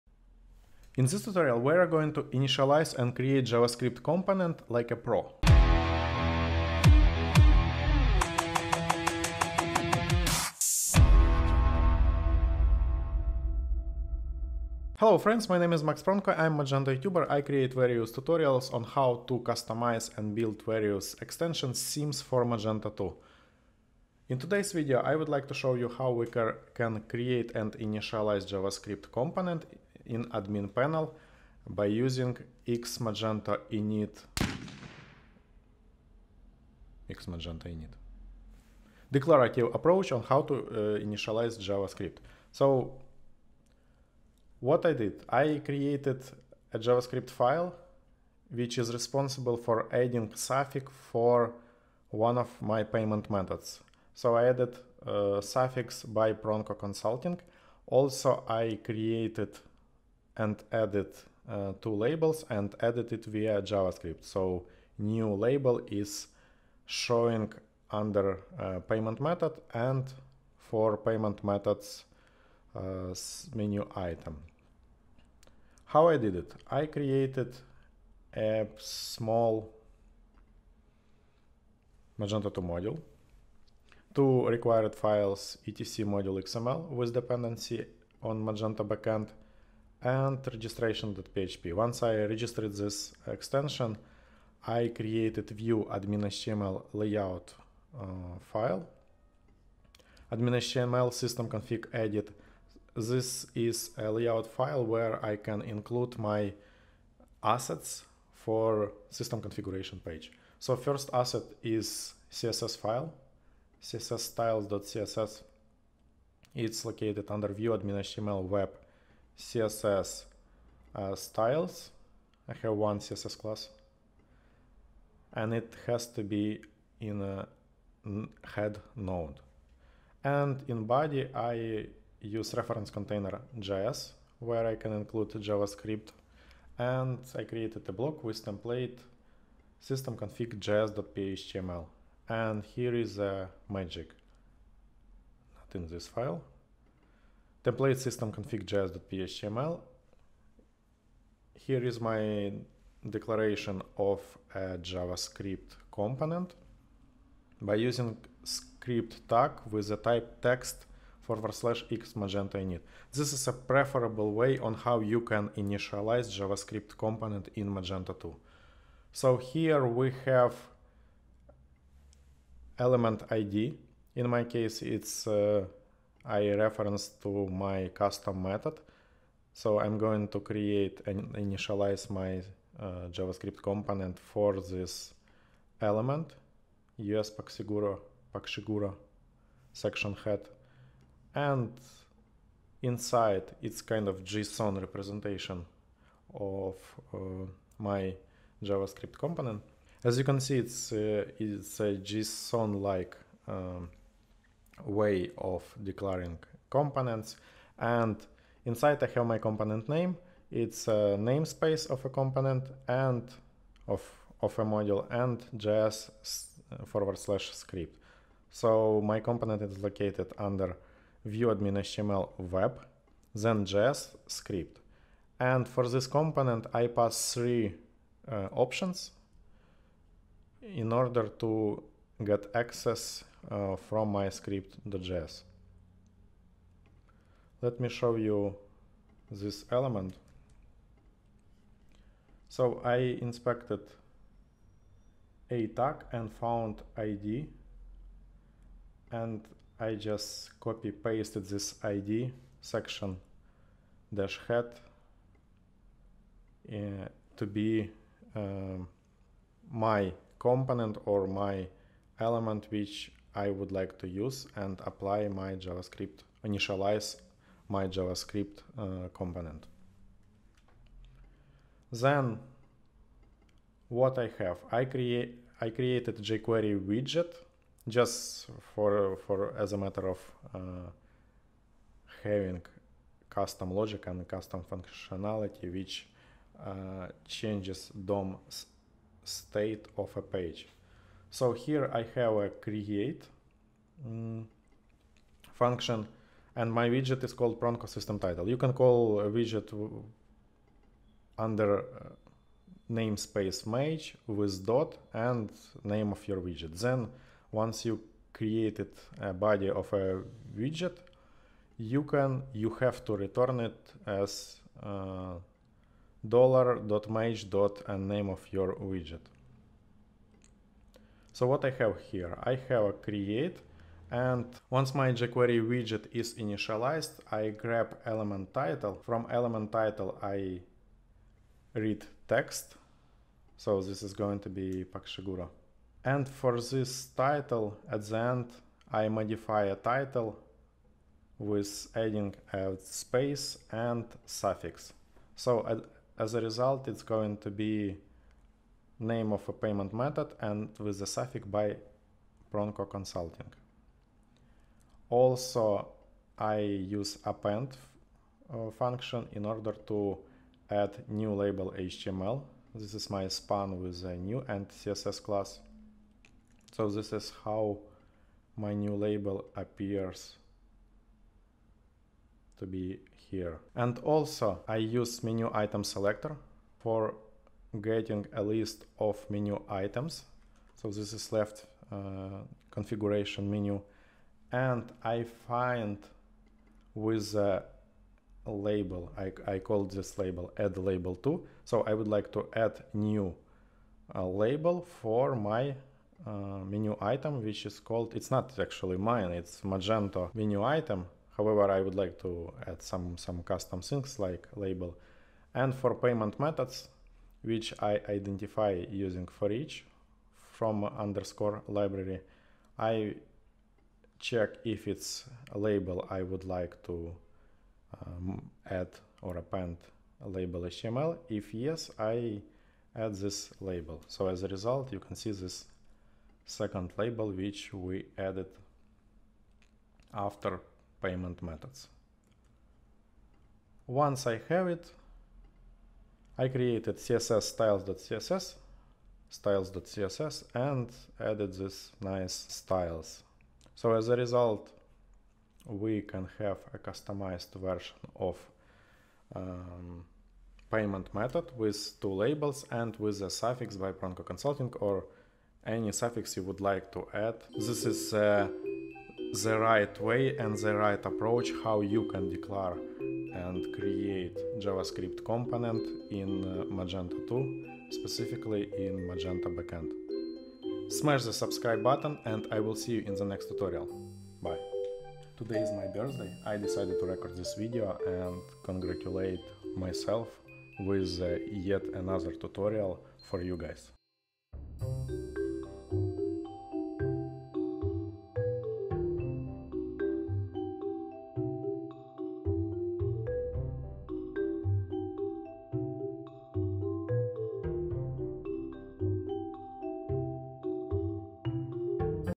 How to initialize JavaScript Component like a PRO | Magento 2
In this tutorial, I will show you how to initialize a JavaScript Component in Magento 2 backend by using text/x-magento-init declarative approach. Also,  I will create jQuery Widget which customizes the look of the configuration settings.